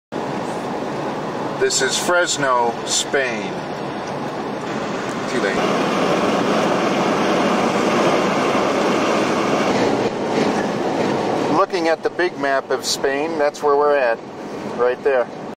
Drive through Fresno, Spain on N-122. 8/30/99.
We drove through Fresno on our way to Bilbao on August 30th, 1999.